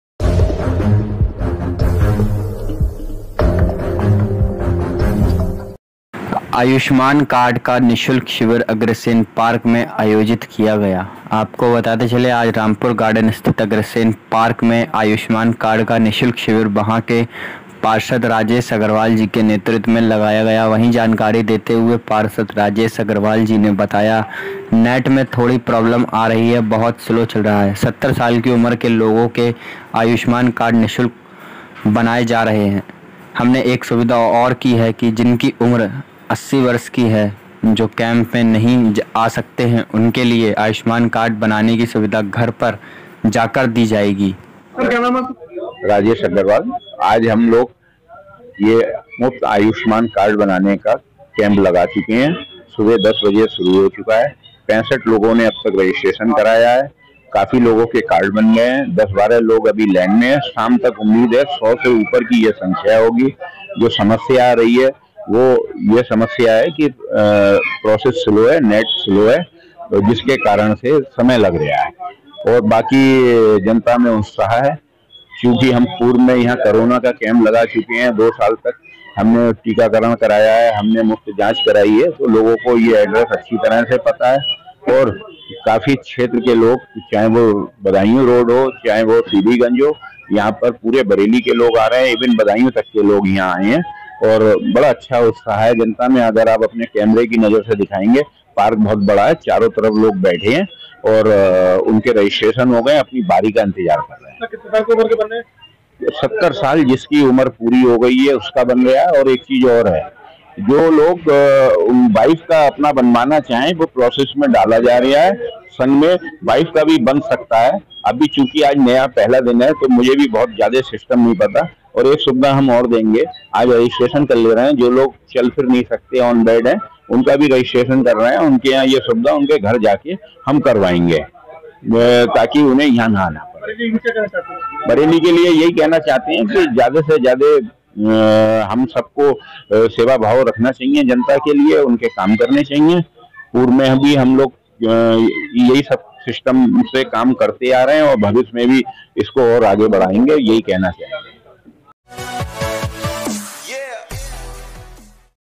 Bareilly-आयुष्मान कार्ड का निःशुल्क शिवर अग्रसेन पार्क में आयोजित किया गया
आयुष्मान कार्ड का निःशुल्क शिवर अग्रसेन पार्क में आयोजित किया गया

बरेली । आज रामपुर गार्डन स्थित अग्रसेन पार्क में आयुष्मान कार्ड का निशुल्क शिविर वहां के पार्षद राजेश अग्रवाल जी के नेतृत्व में लगाया गया वही जानकारी देते हुए पार्षद राजेश अग्रवाल जी ने बताया नेट में थोड़ी प्रॉब्लम आ रही है बहुत स्लो चल रहा है 70 साल की उम्र के लोगों के आयुष्मान कार्ड निशुल्क को बनाए जा रहे हैं हमने एक सुविधा और की है जिनकी उम्र 80 वर्ष की है जो कैंप में नहीं आ सकते हैं उनके लिए आयुष्मान कार्ड बनाने की सुविधा घर पर जाकर दी जाएगी।